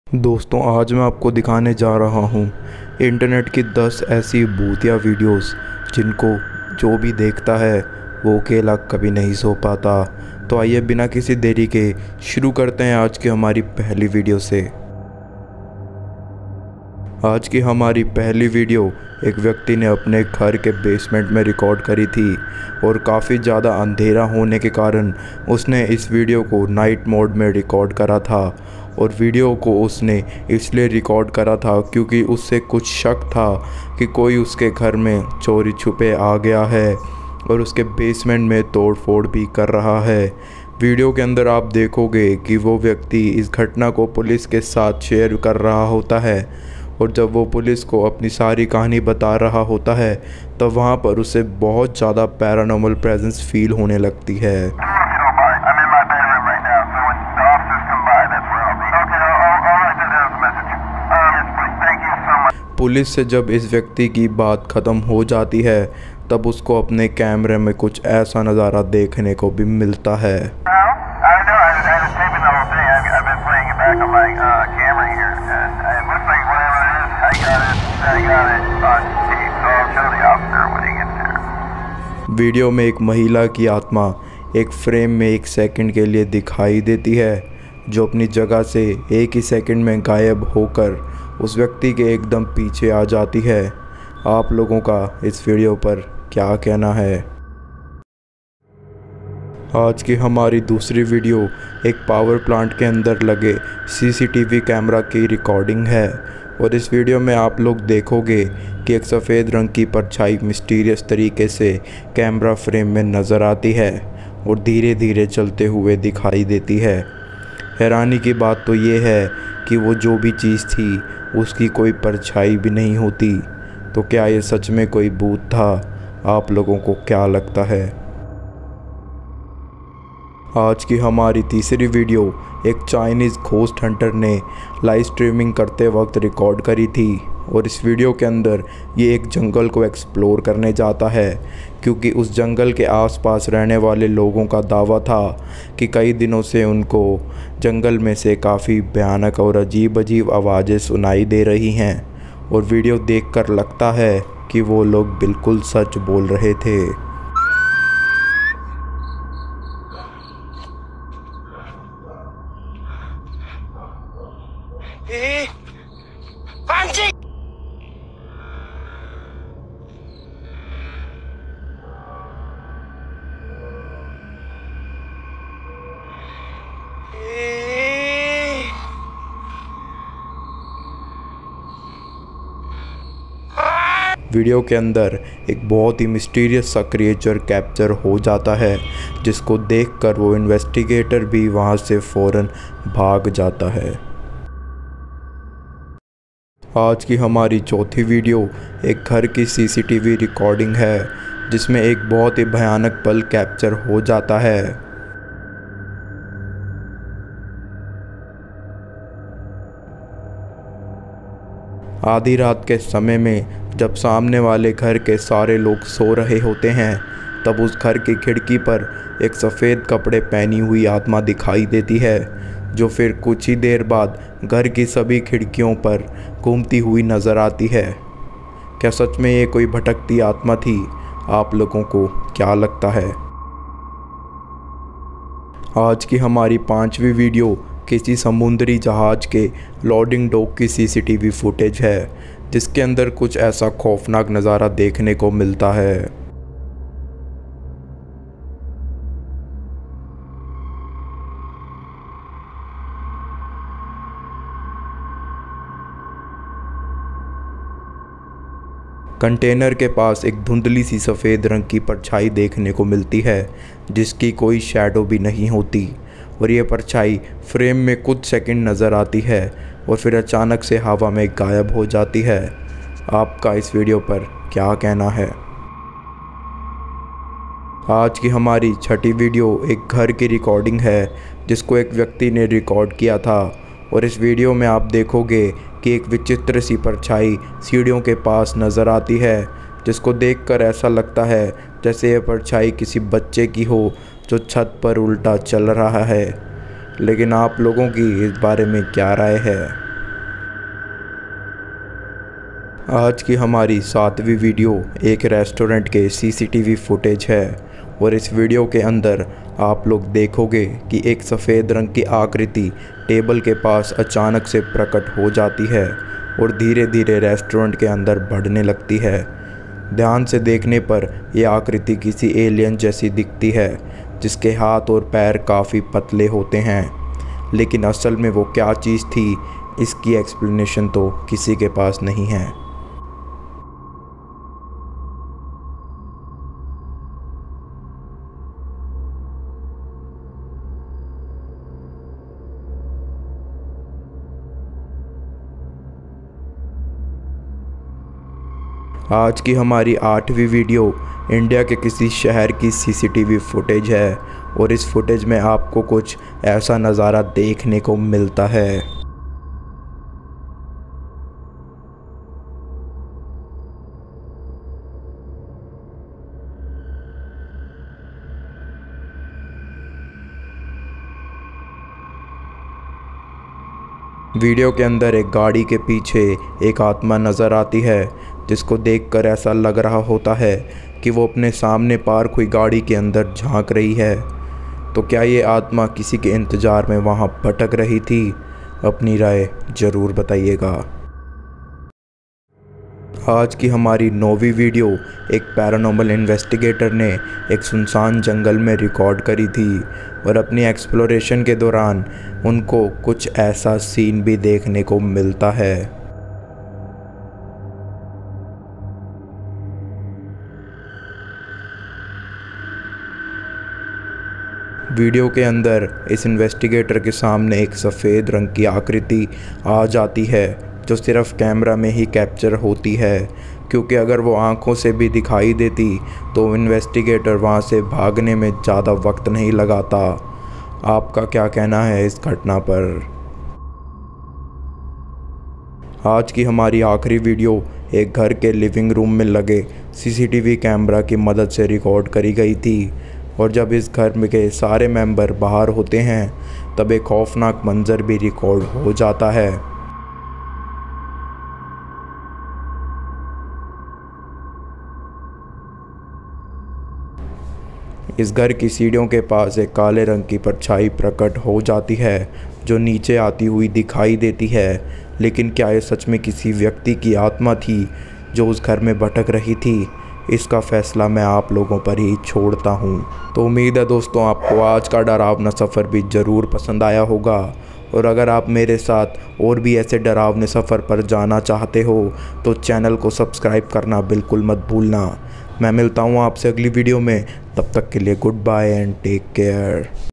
Top 10 Real Scary Ghost Videos If Not Recorded You Would Never Trust (Hindi)
Top 10 Real Scary Ghost Videos If Not Recorded You Would Never Trust

Get Ready To Get Chills Down Your Spine. After Watching This Horror Video I Guarantee You Will Never Want To Sleep Alone Or Go To The Bathroom

Copyrights :- ScaryPills